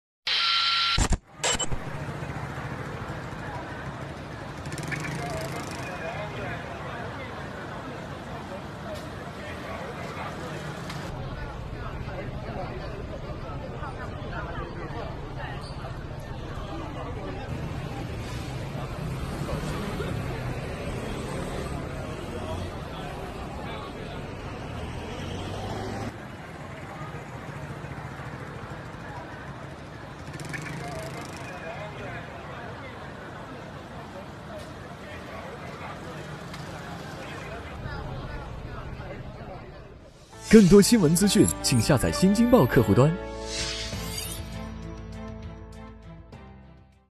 台湾地震致大楼倾斜# 台灣花蓮地震致大樓傾斜
【台湾地震致大楼倾斜# 楼内人员全部疏散】4月18日，台湾花莲6.7级地震#，全台湾均有震感。台北市长安东路大楼倾斜，靠到隔壁大楼，外墙瓷砖掉落。该大楼共12层，楼龄约36年，楼内人员已全部撤出。警方封闭临近路段，呼吁民众不要围观。